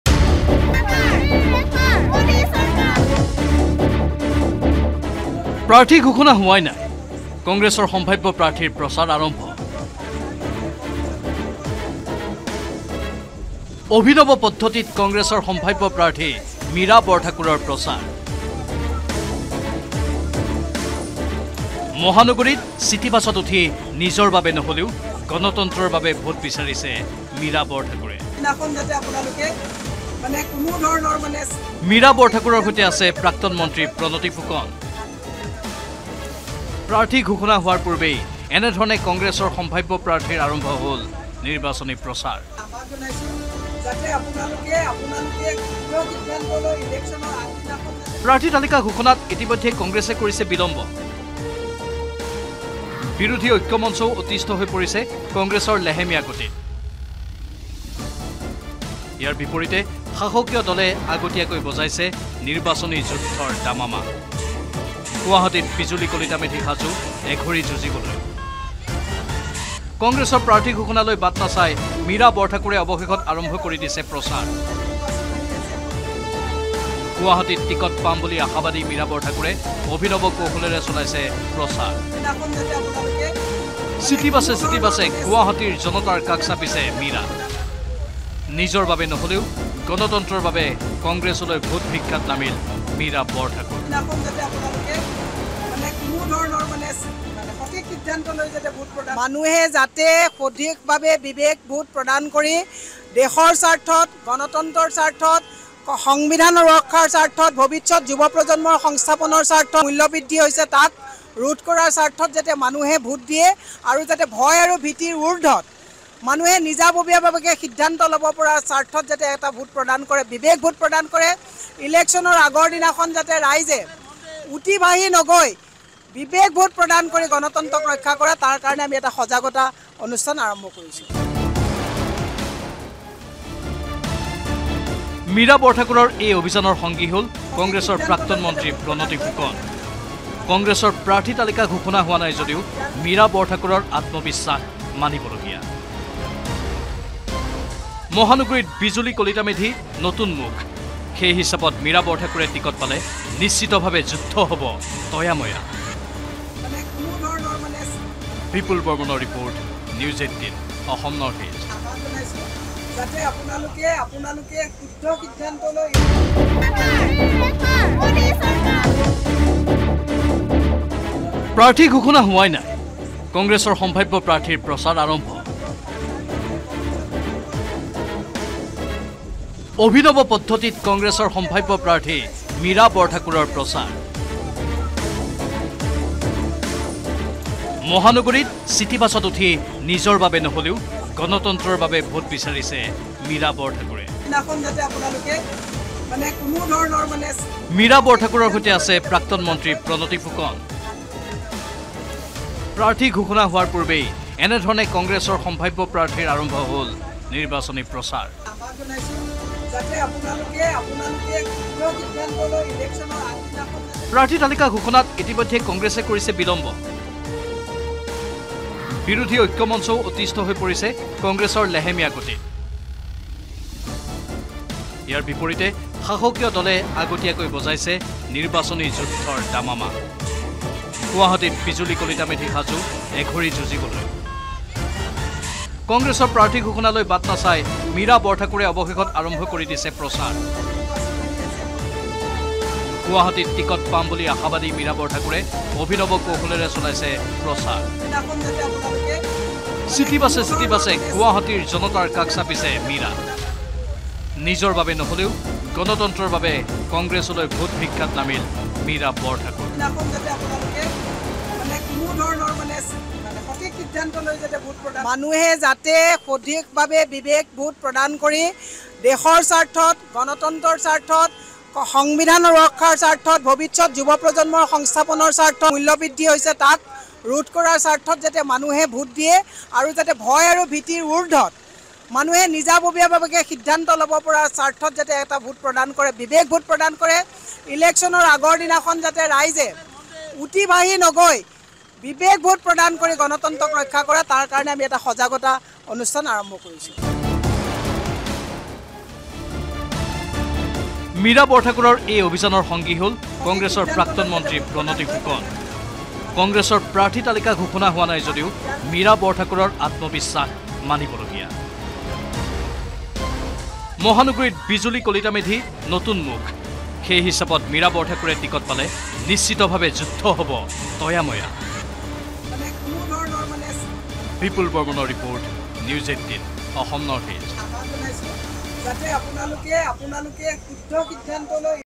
Mira Borthakur Election Campaign | কংগ্ৰেছৰ সম্ভাব্য প্ৰাৰ্থীৰ প্ৰচাৰ আৰম্ভ। N18V
Mira Borthakur Election Campaign |  প্ৰাৰ্থী ঘোষণা হোৱাই নাই , কংগ্ৰেছৰ সম্ভাব্য প্ৰাৰ্থীৰ প্ৰচাৰ আৰম্ভ।

অভিনৱ পদ্ধতিত কংগ্ৰেছৰ সম্ভাব্য প্ৰাৰ্থী মীৰা বৰঠাকুৰৰ প্ৰচাৰ।

মহানগৰীত চিটি বাছত উঠি নিজৰ বাবে নহলেও গনতন্ত্ৰৰ বাবে ভোট বিচাৰিছে মীৰা বৰঠাকুৰে।

মীৰা বৰঠাকুৰৰ সৈতে আছে প্ৰাক্তন মন্ত্ৰী প্ৰনতী ফুকন